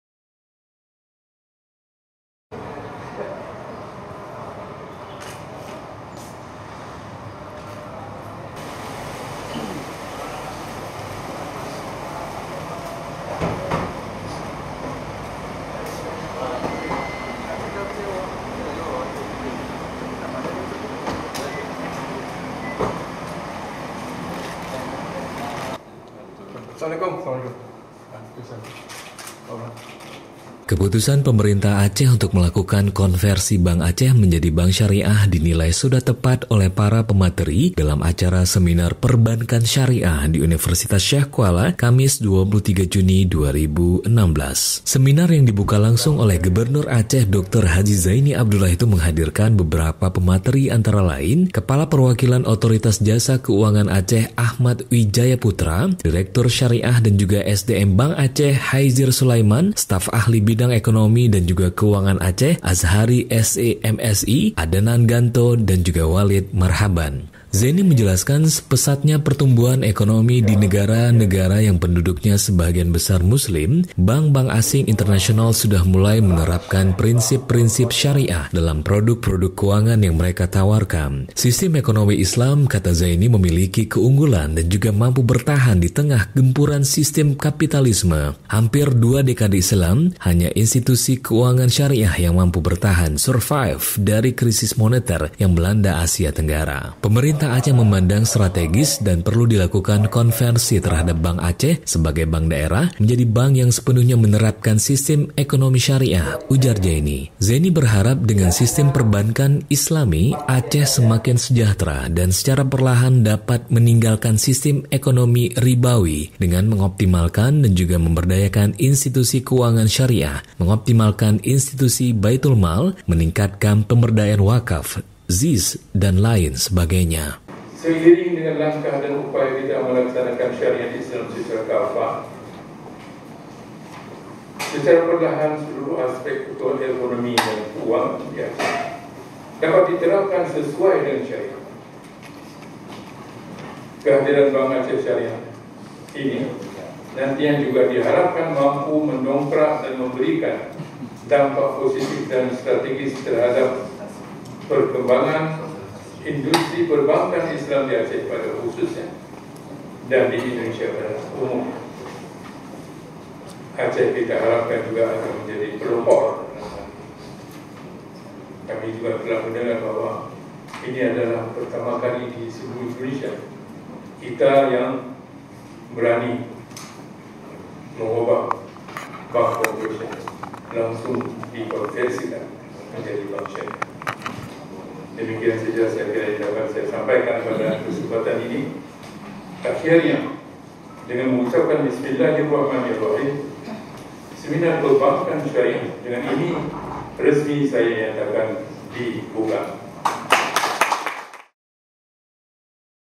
GUBERNUR BUKA SEMINAR PERBANKAN SYARIAH
Banda Aceh – Keputusan Pemerintah Aceh untuk melakukan konversi Bank Aceh menjadi Bank Syariah dinilai sudah tepat. Hal itu disebutkan dalam Seminar Perbankan Syariah di Universitas Syiah Kuala, Kamis, (23/6).

Seminar yang dibuka langsung oleh Gubernur Aceh, dr. H. Zaini Abdullah itu menghadirkan beberapa pemateri antara lain, Kepala Perwakilan Otoritas Jasa Keuangan Aceh, Ahmad Wijaya Putra, Direktur Syariah dan SDM Bank Aceh, Haizir Sulaiman, Staf Ahli Bidang Ekonomi dan Keuangan Aceh, Azhari, SE, M.Si, Dr. Sabri Abdul Majid,  Adnan Ganto, dan Waled Marhaban.